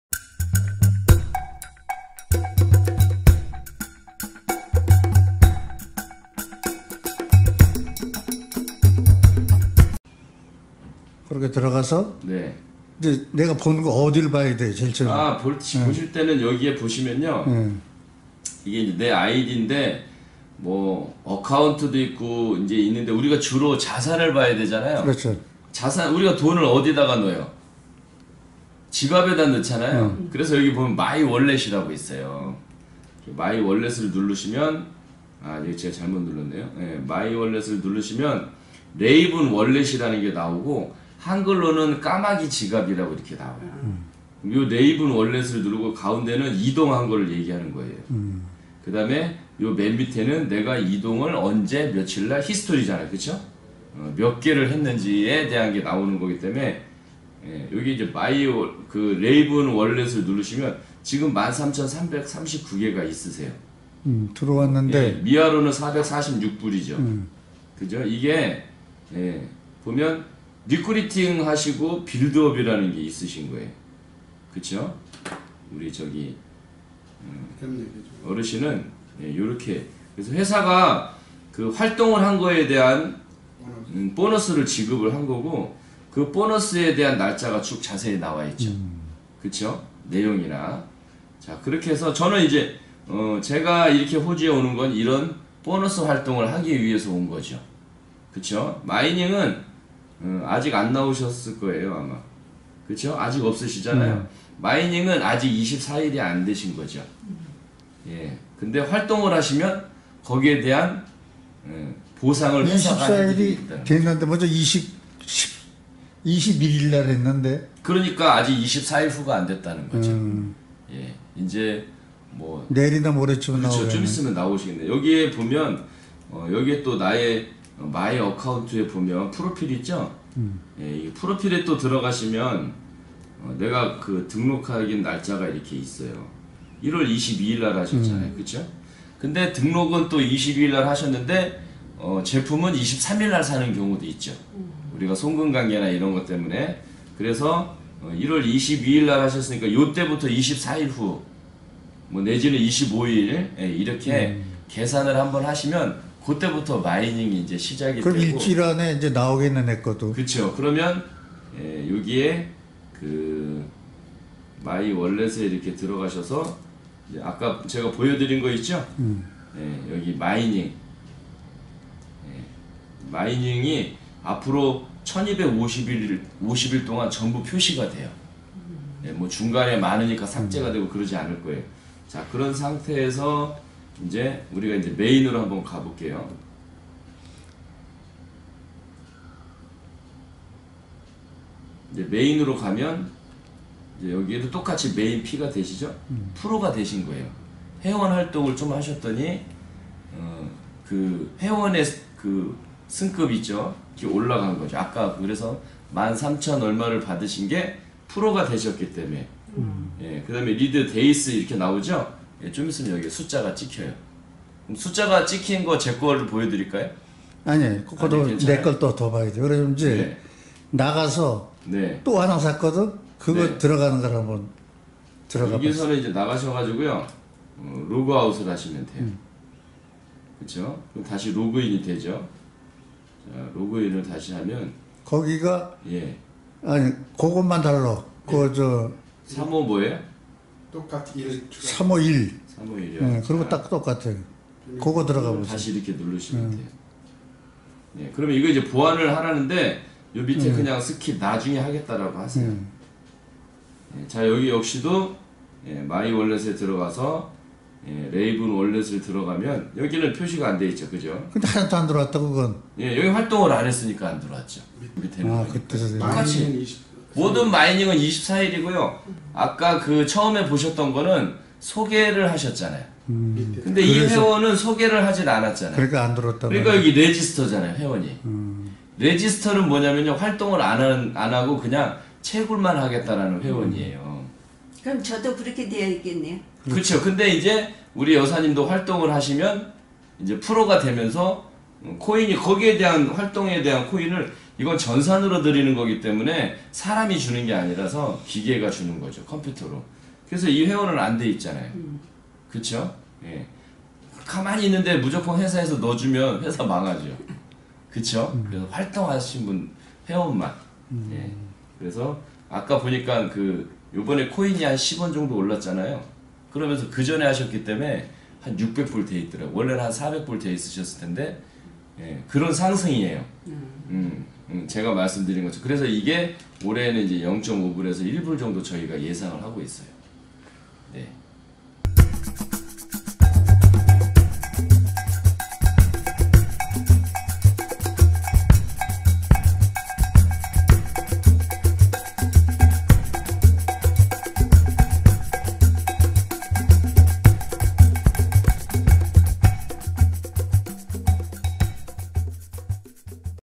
레이본 코인 정우철 강사와의 1문 1답 제3화
레이본 코인에 대한 궁금한 내용을 정유철 강사와의 1문 1답을 통해 해결해 드리는 시리즈 영상입니다.